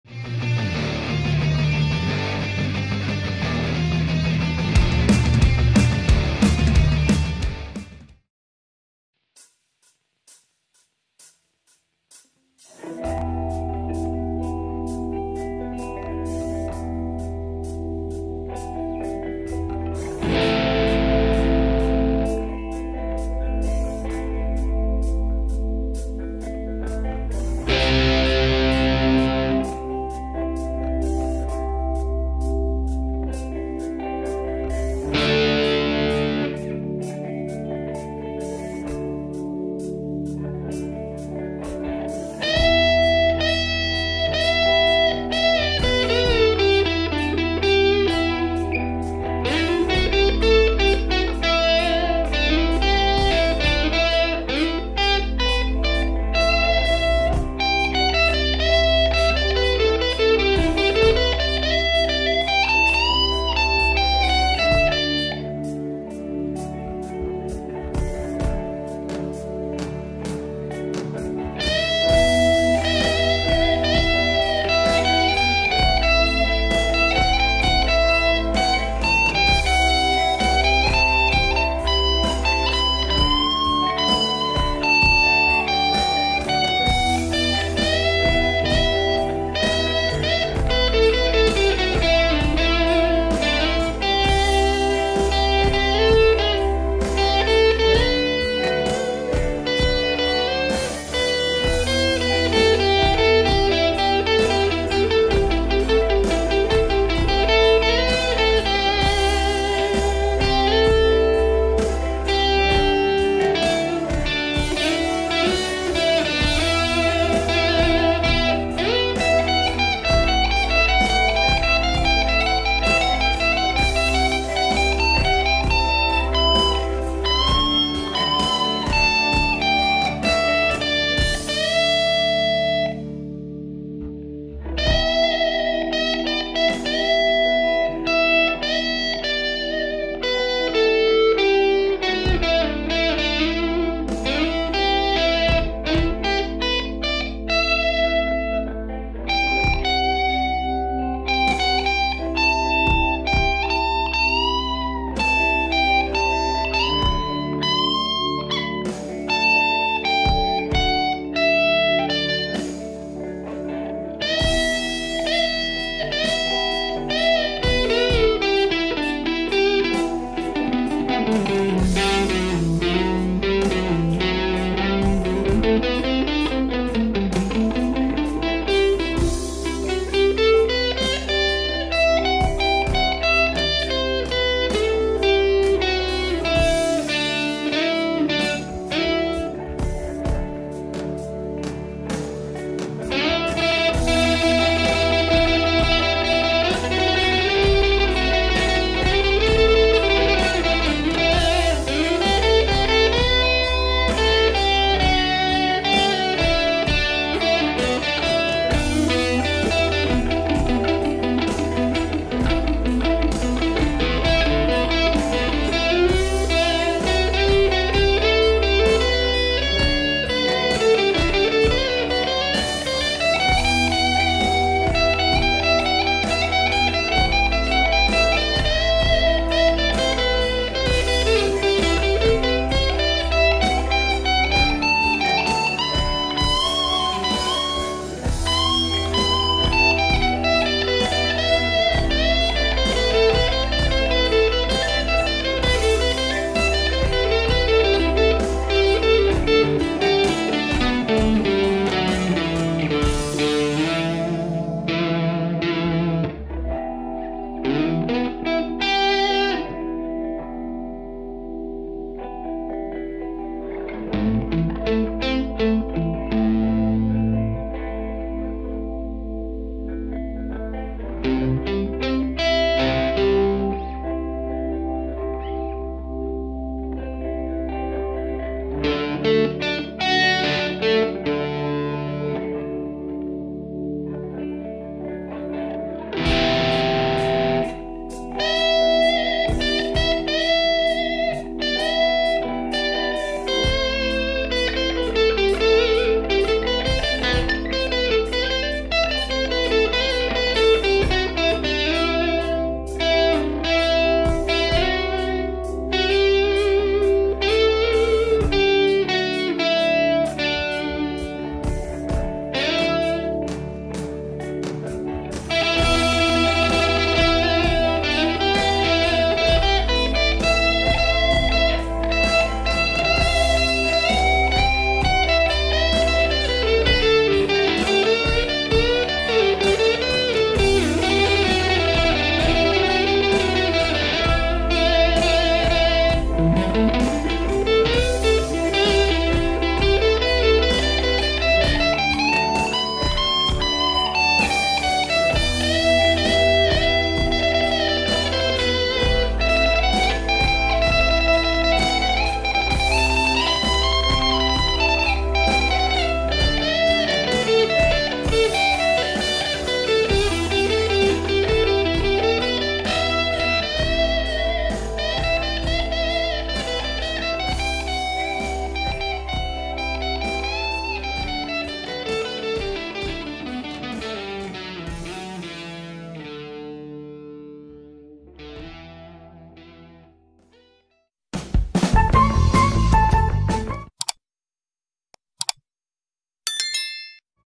E MINOR SAD BALLAD
Hi all.
I'm playing against a E minor backing track from Nick Neblo
I used the Ignite Emissary bundle in Reaper.
I used my 2020 Gibson Sg.
This is a great slow backing track to practice to.